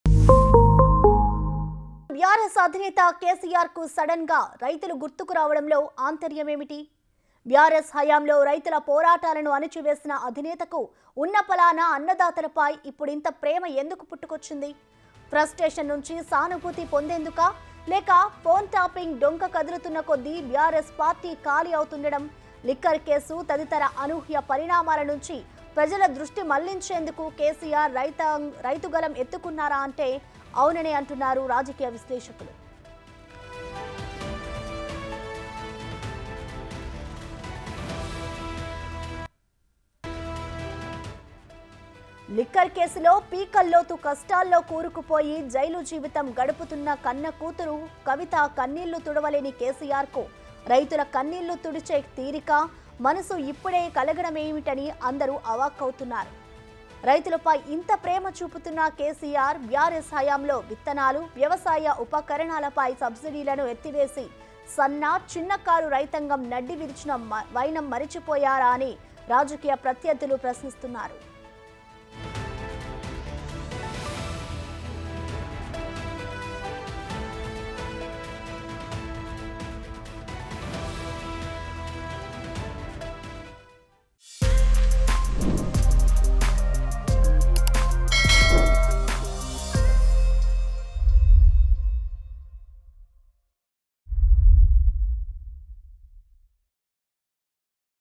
KCR diversion politics: కన్న కూతురు కన్నీళ్లు తుడవలేని కేసీఆర్.. రైతుల కన్నీళ్లు తుడుస్తాడా? | BIGTV
What is the meaning of BRS chief KCR suddenly remembering the farmers? Why did the leader who suppressed farmers' struggles during the BRS regime have so much love for rice farmers? Political analysts say yes, to get sympathy from frustration.. or to divert people's attention from unexpected developments like the liquor case, BRS party is emptying out as the phone tapping detour moves.